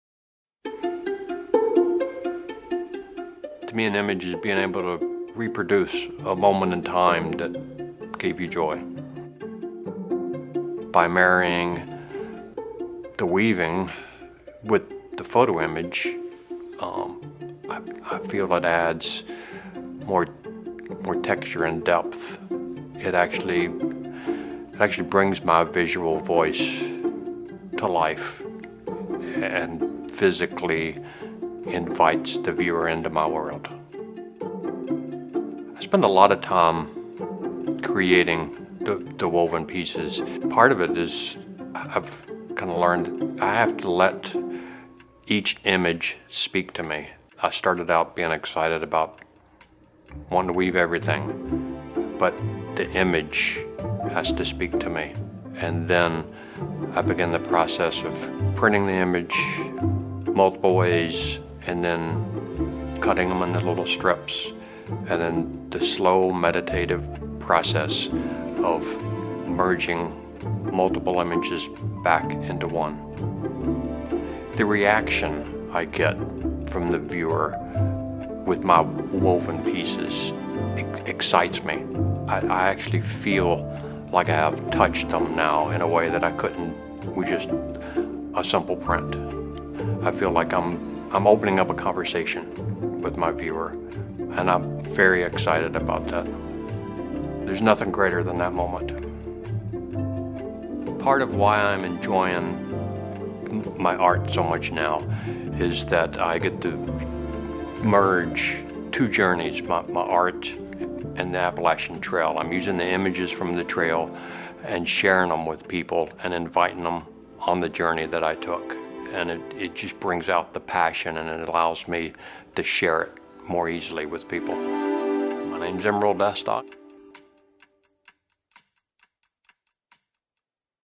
Emerald Estock - Weaving art
Emerald Estock is a photographer/artist/AT Thru Hiker who found new inspirations for his photography thru basket weaving